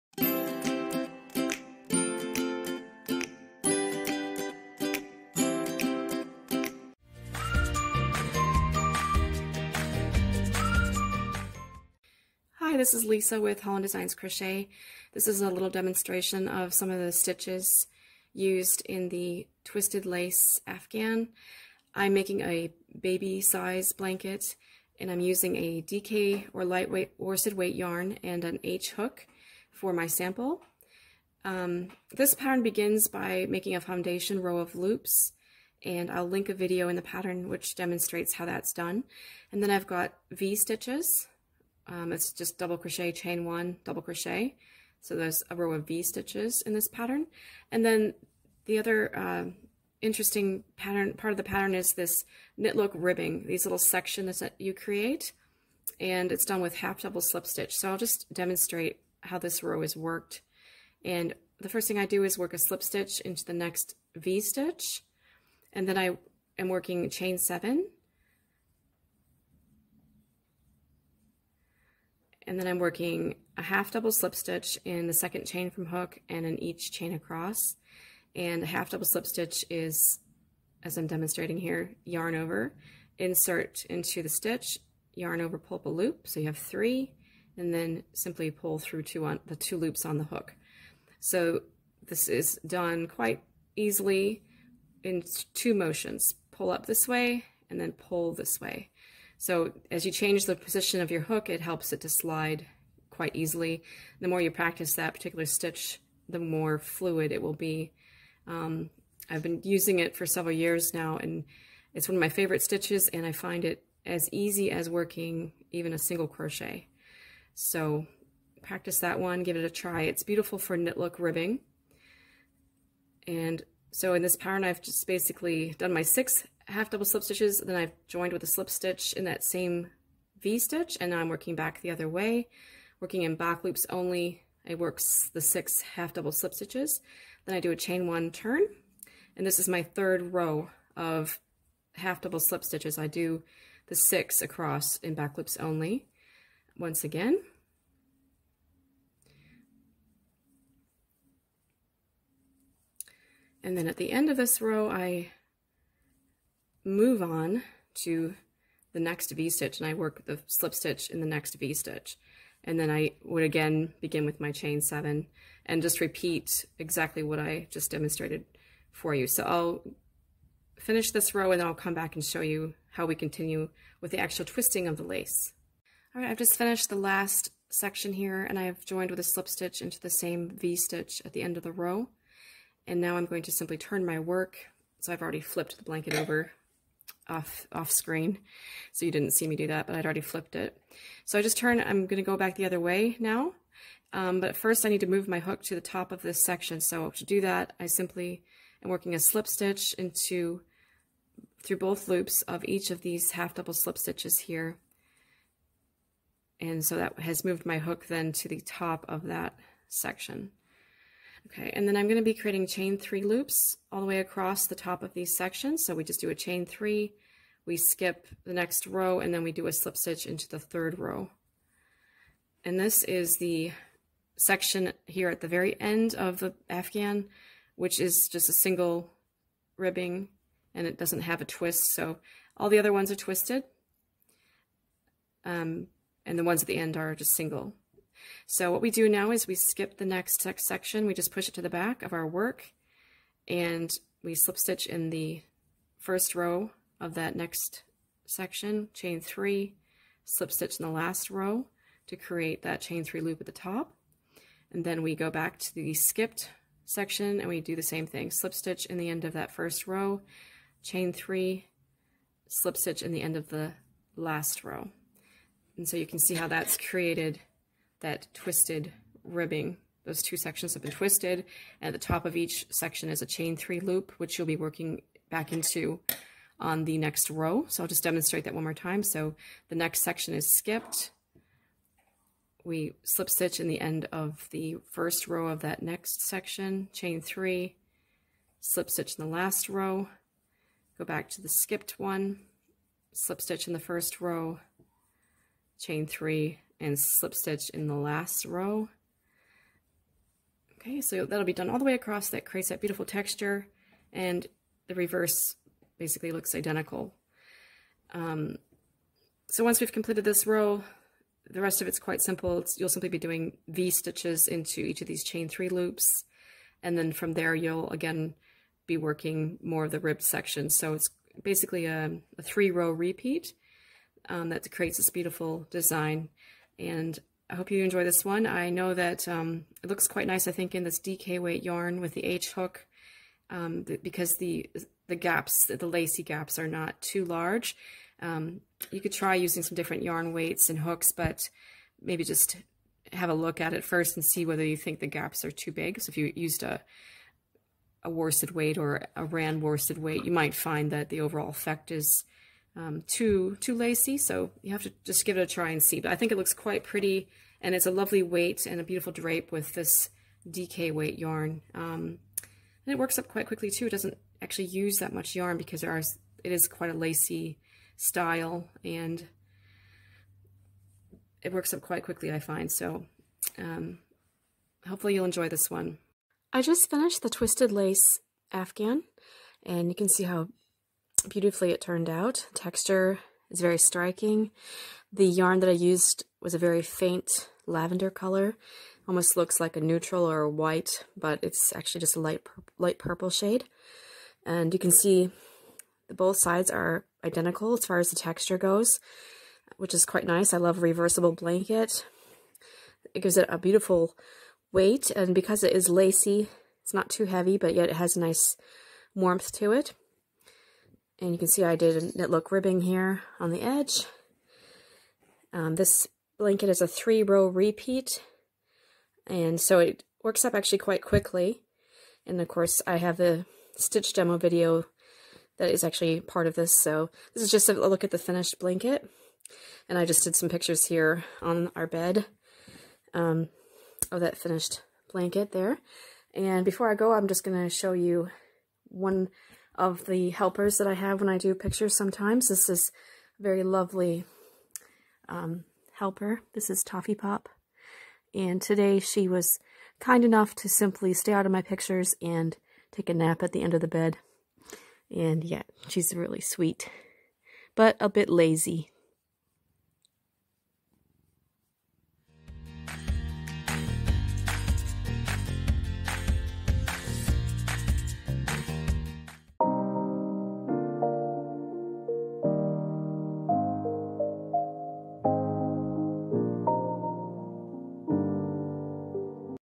Twisted Lace Afghan Crochet Stitch Demo
The Twisted Lace Afghan is a beautiful, reversible, knit-look textured blanket. It may be made to any desired dimensions. The stitch pattern is only a 3-row repeat and works up quite quickly.